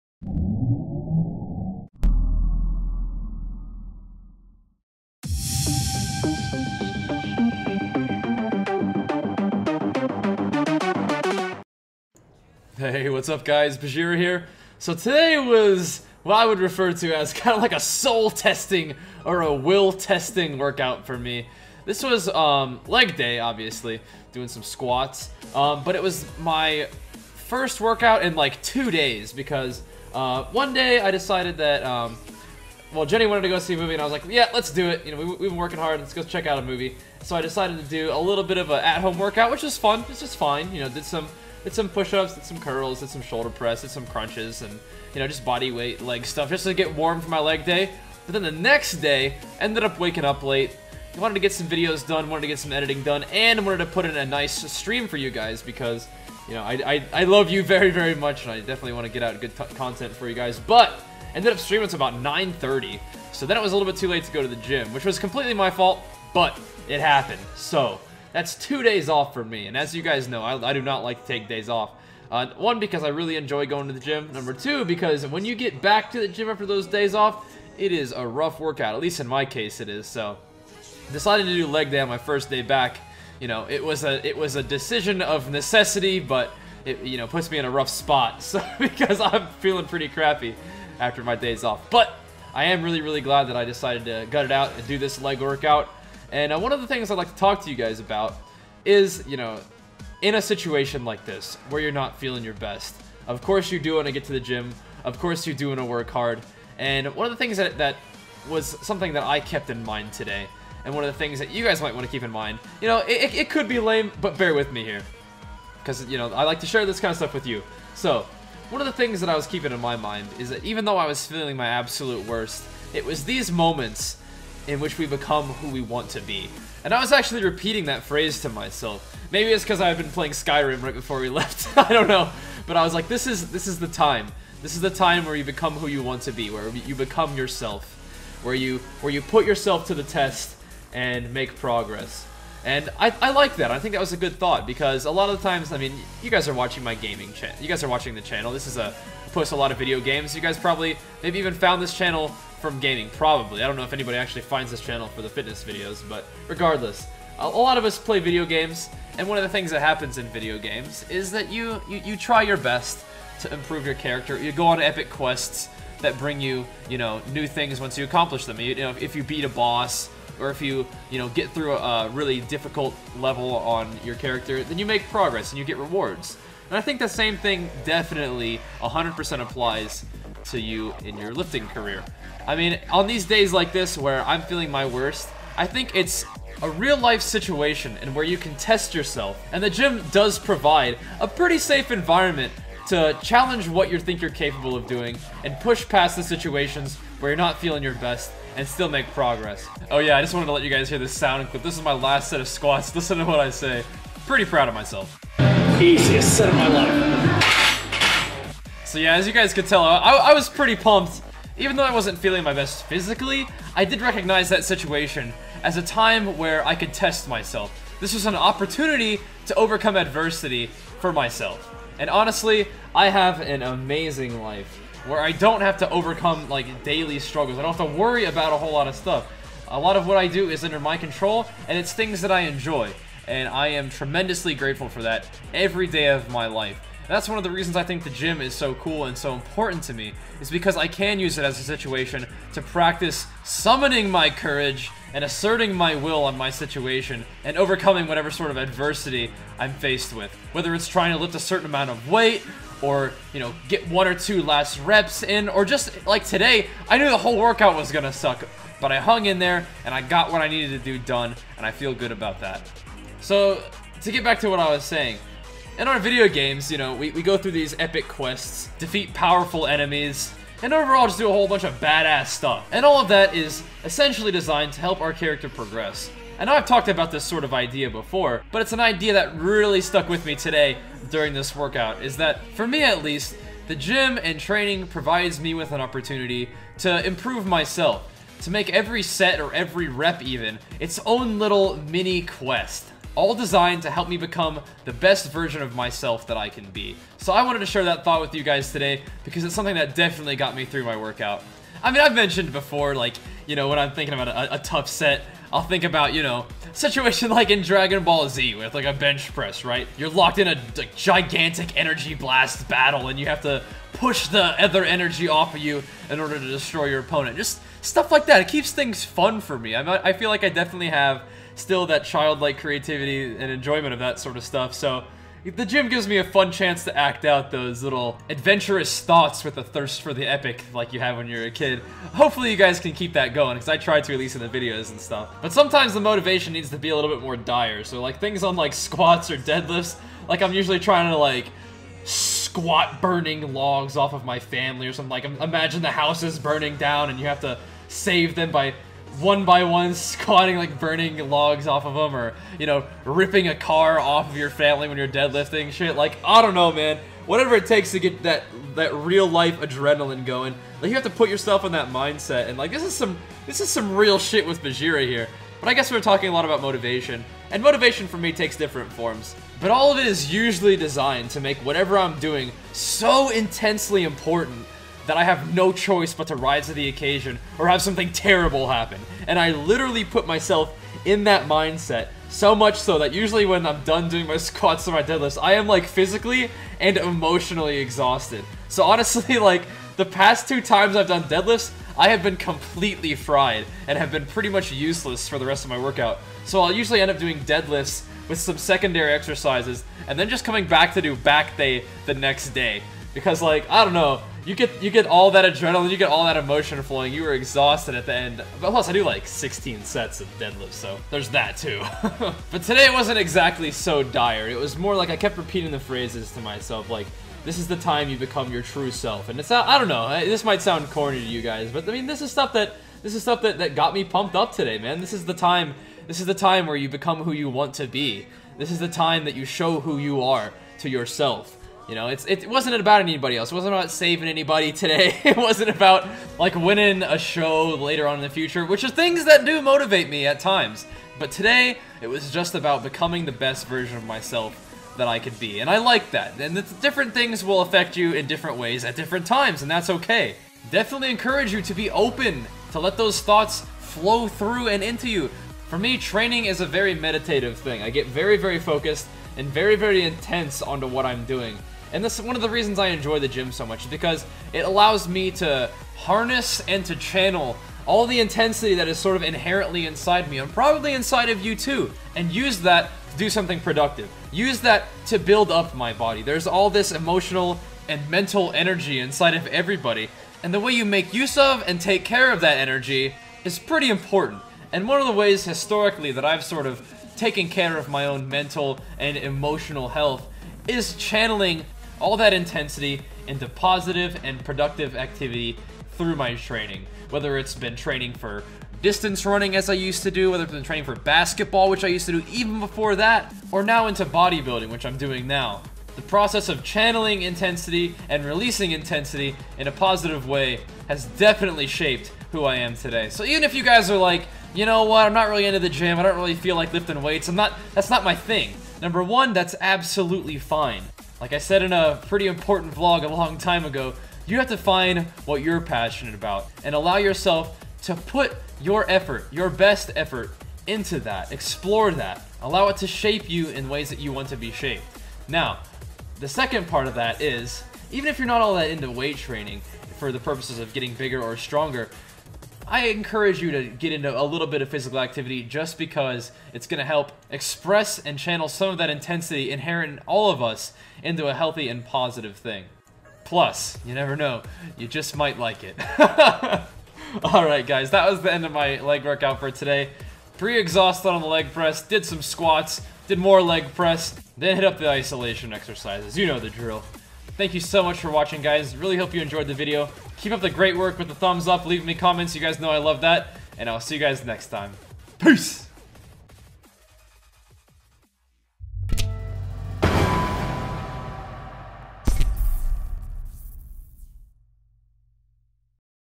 BajheeraIRL - Leg Day w/ Motivation & Intensity Discussion - Natural Bodybuilding Vlog
Sup guys, posting some of my Leg Workout today for you guys to check out along with some thoughts about Motivation, Intensity, and how they interact :D

CELLUCOR: Results Matter - Award Winning Fitness Supplements